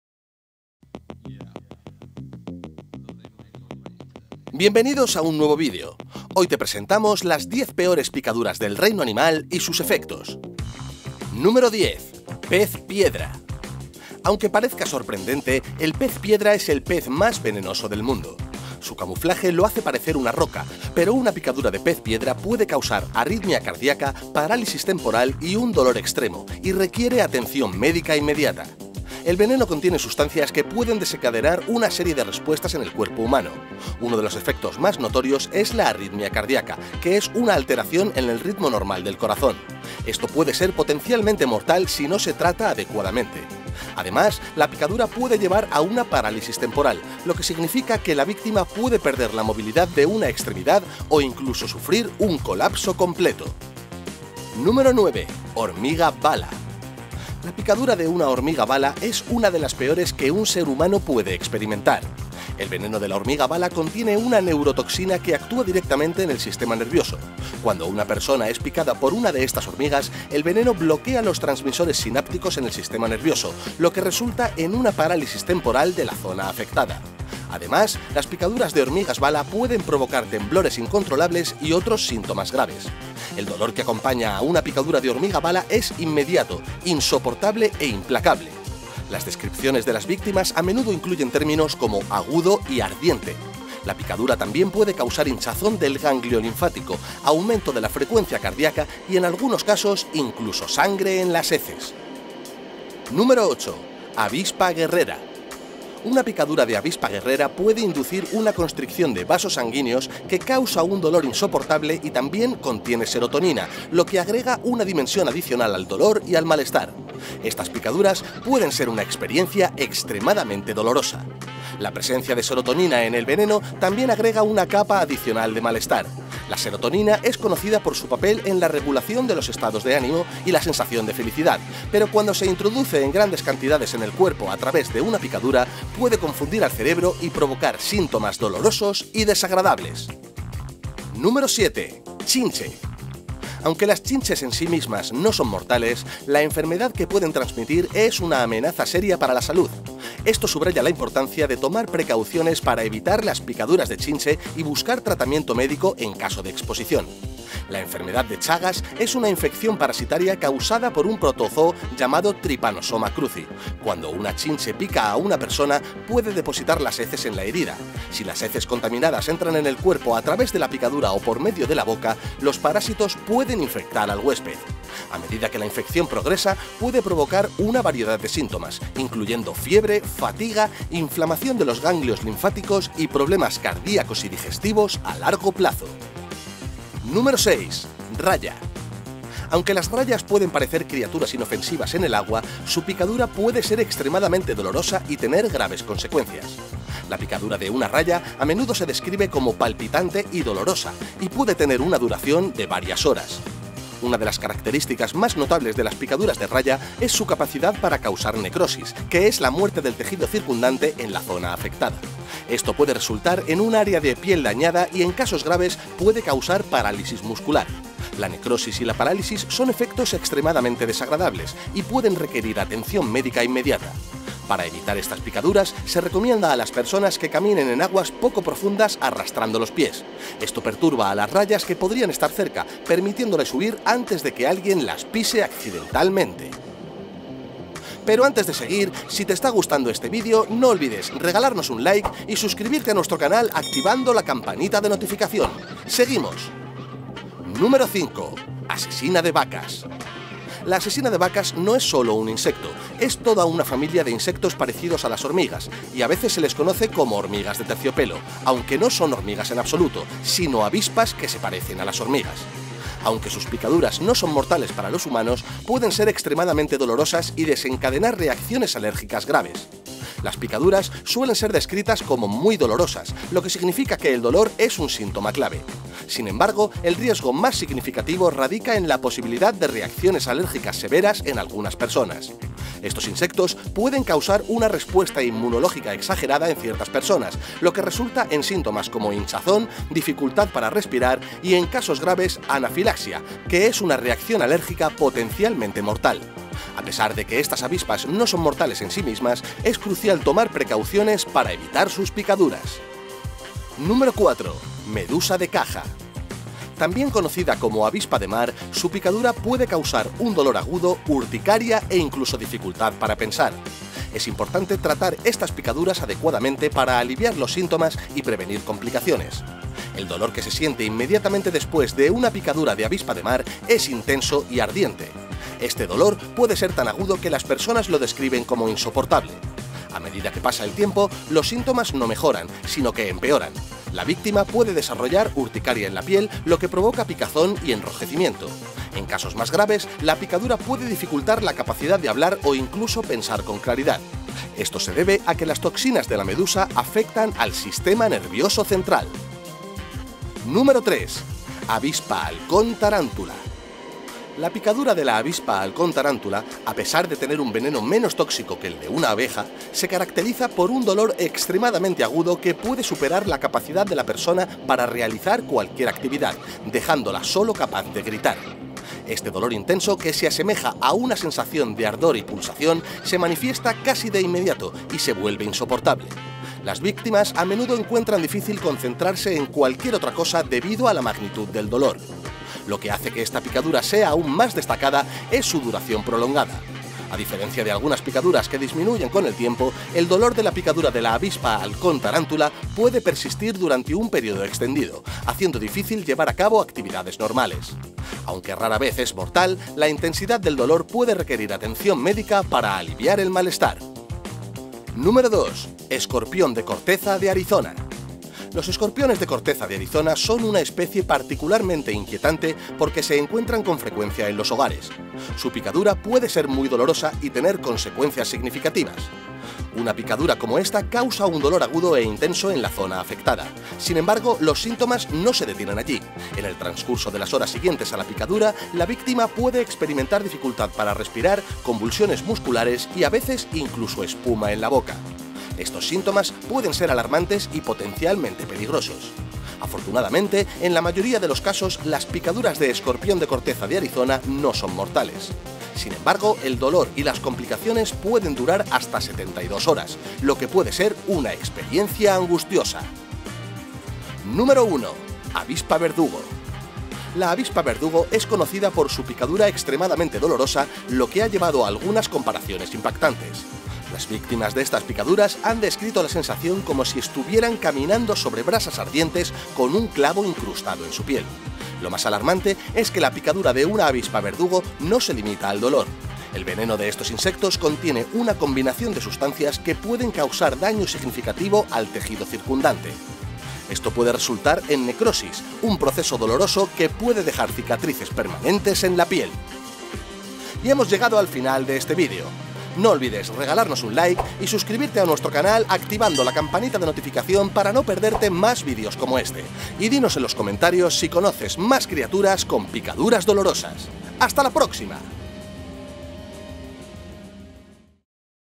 Las PICADURAS Más Impactantes Y Devastadoras Que Debes Evitar
¡Prepárate para un video impactante y lleno de información crucial! En este emocionante video de YouTube, te presentamos las diez picaduras más impactantes y devastadoras que debes evitar a toda costa.

Exploraremos el mundo de los animales venenosos y conoceremos a las criaturas más temidas por sus potentes picaduras. Desde el pez piedra hasta la hormiga bala y las chinches, descubrirás cómo estas picaduras pueden resultar extremadamente dolorosas y, en algunos casos, incluso mortales.

A través de imágenes de alta calidad y testimonios de expertos en vida silvestre, te adentrarás en la naturaleza salvaje y aprenderás sobre los efectos devastadores que pueden tener estas picaduras en el cuerpo humano. Conocerás los síntomas, las precauciones que debes tomar y las medidas de primeros auxilios necesarias en caso de sufrir una de estas picaduras.

¡No te pierdas este fascinante video que te mantendrá al filo de tu asiento mientras descubres las diez picaduras más impactantes y devastadoras que debes evitar a toda costa!